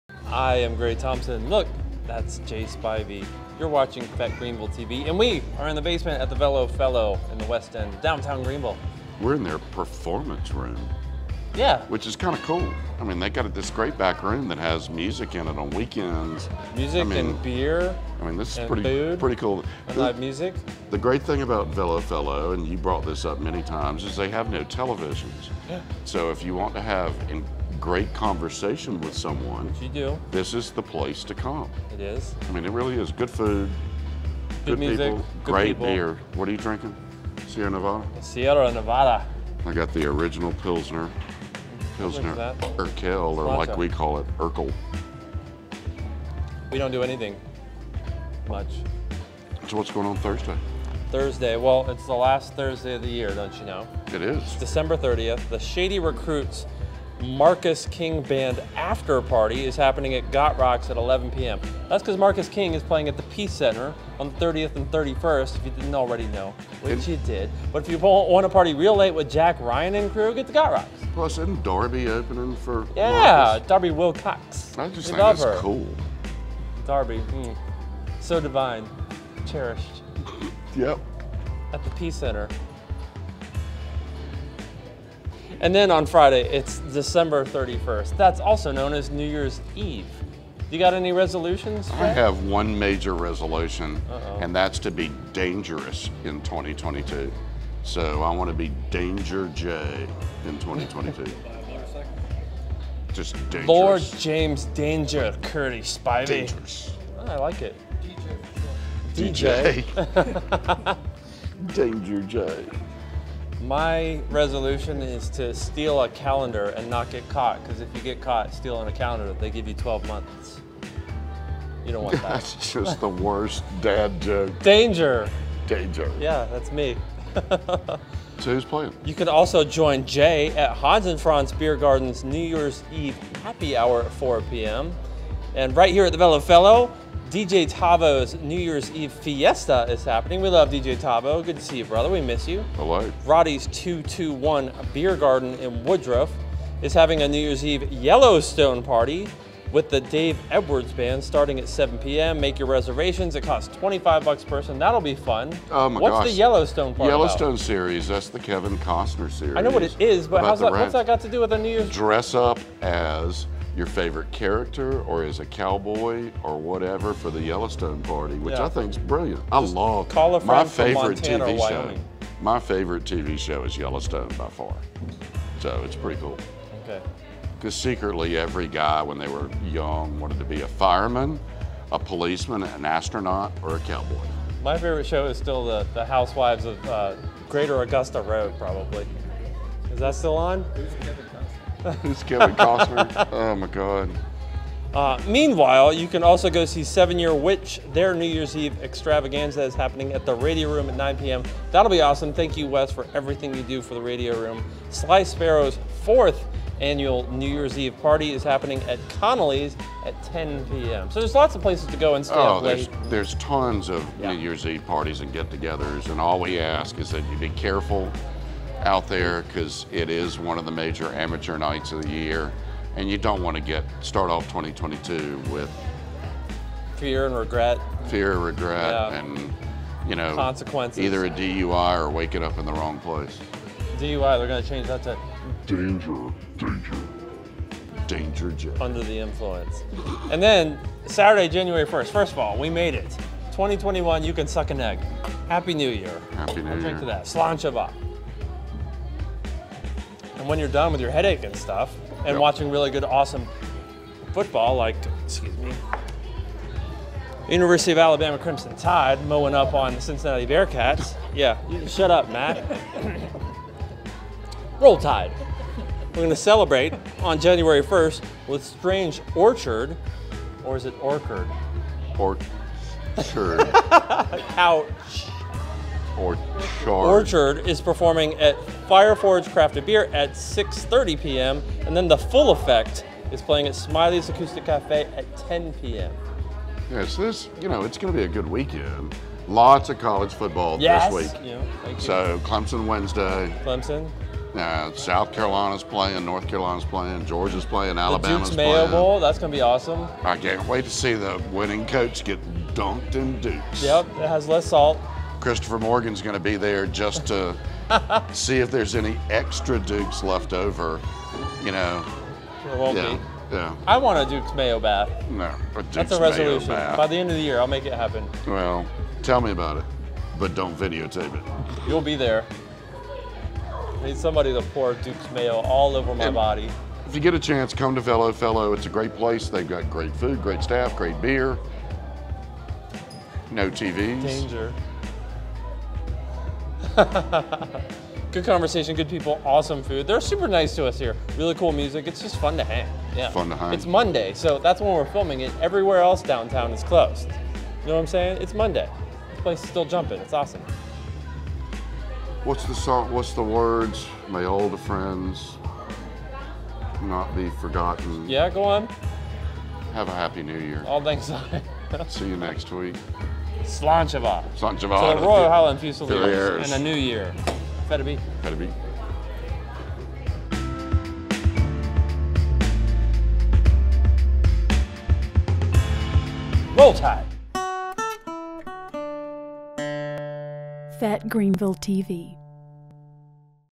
fêteGreenvilleTV's fun stuff to do - Dec. 30 to Jan. 1
Happy New Year! Be safe and have fun this weekend. FGTV was filmed at The Velo Fellow. Go see 'em and help us support our local businesses, artists and musicians around Greenville, SC. fête it be...